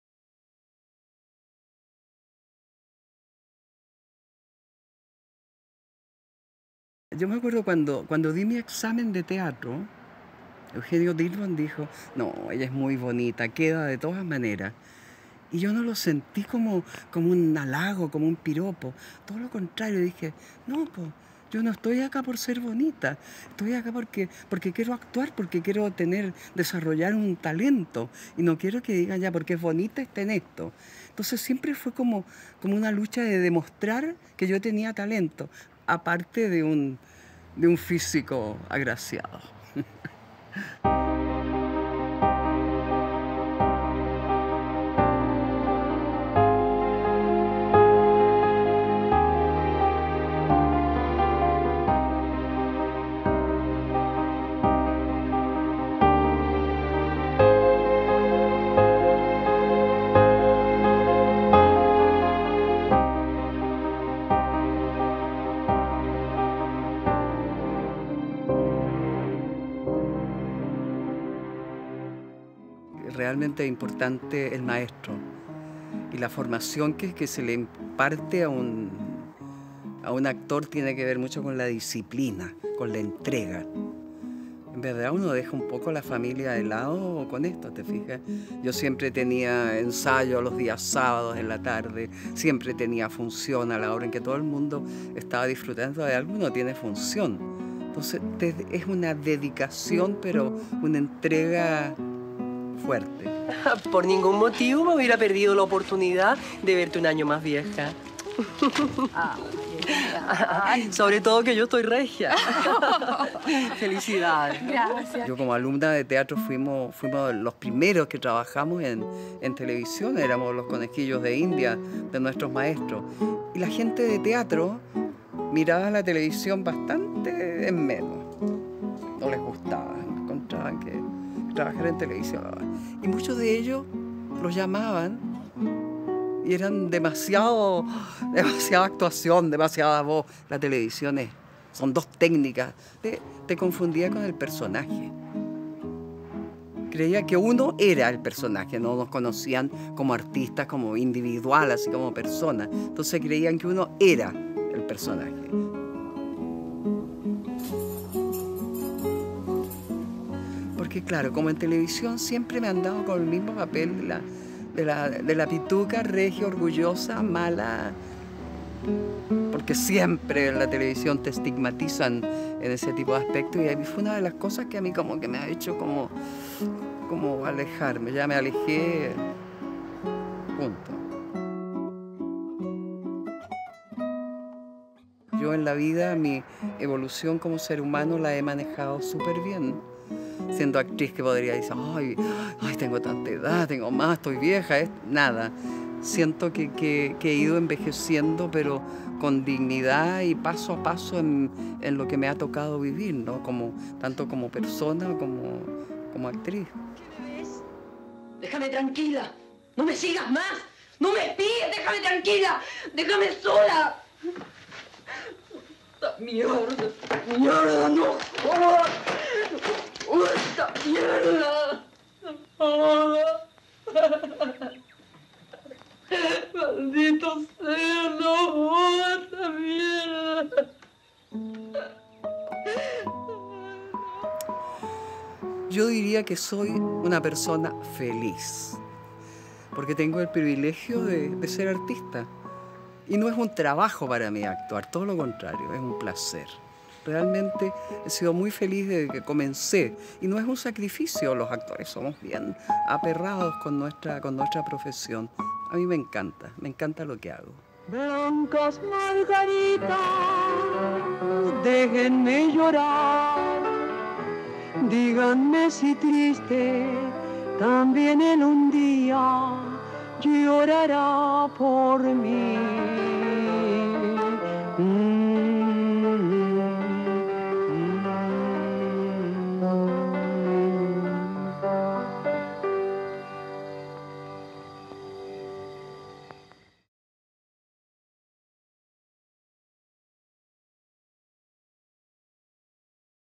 ABCD DIARIO ACTORAL 3: SILVIA SANTELICES
¿Son encasilladas las actrices en televisión? En este capítulo de ABCDiario Actoral 3, nuestra querida compañera Silvia Santelices reflexiona sobre el tema y nos cuenta su valiosa experiencia actoral.

Cápsula audiovisual realizada por Hernán Caffiero y TRIDI 3D Producciones.